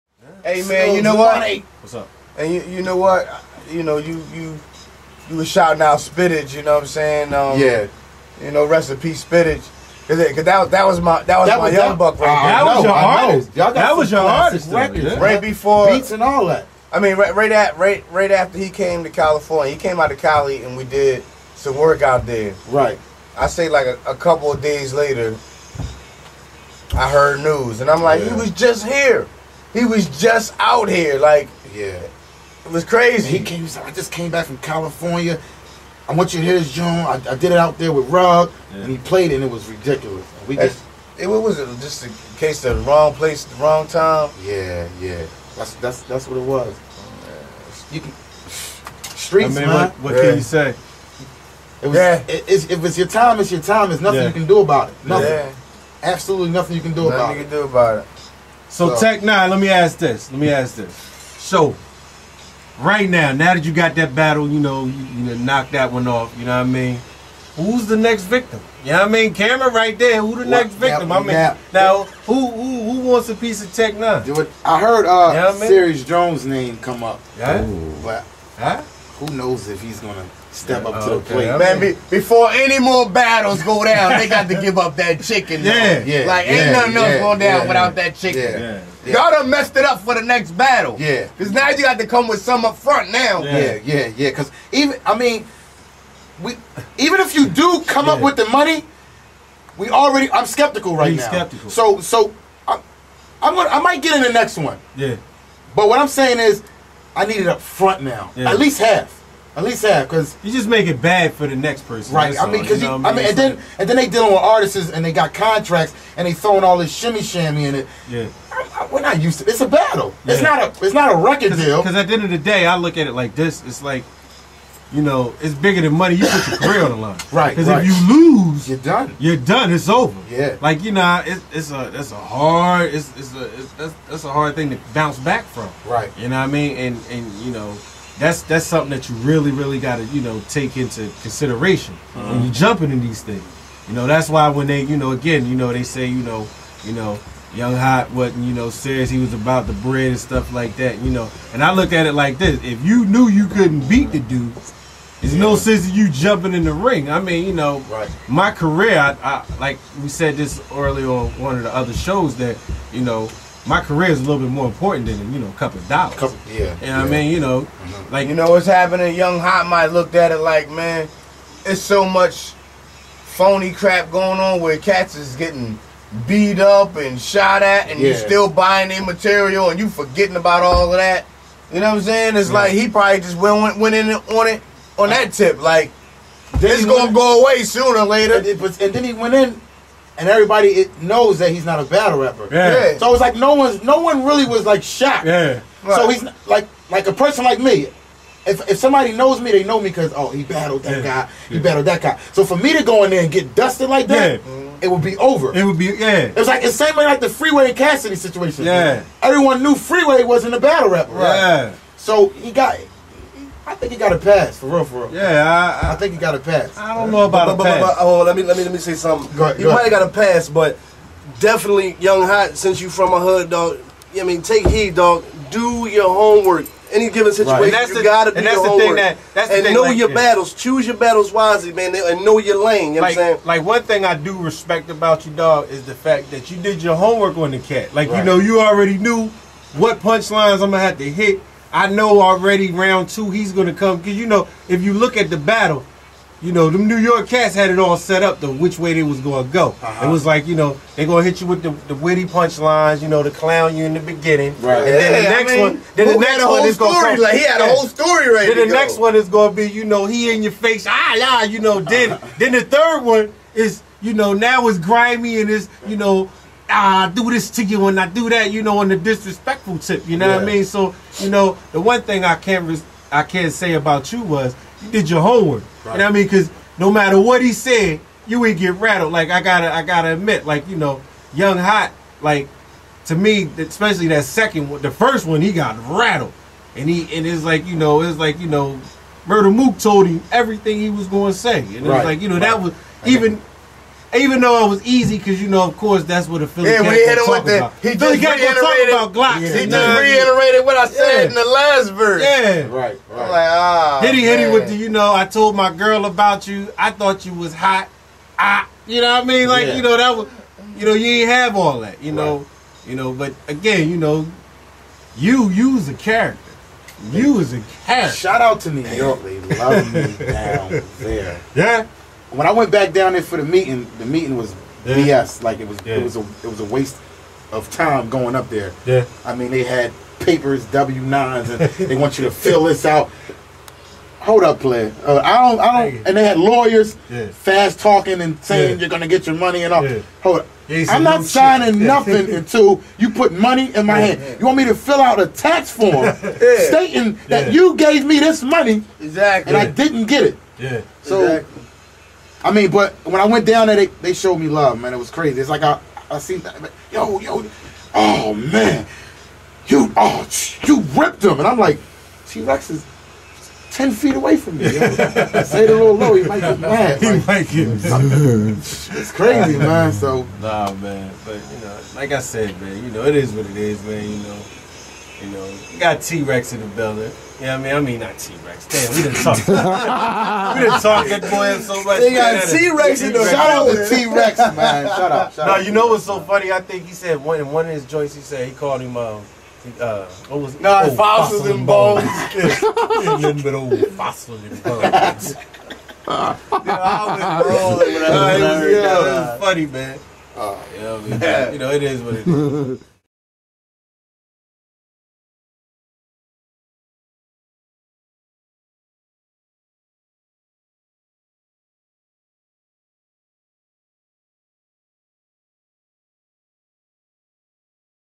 Tech 9 Interview 2 of 2
part2 talks on rumors of Serious Jones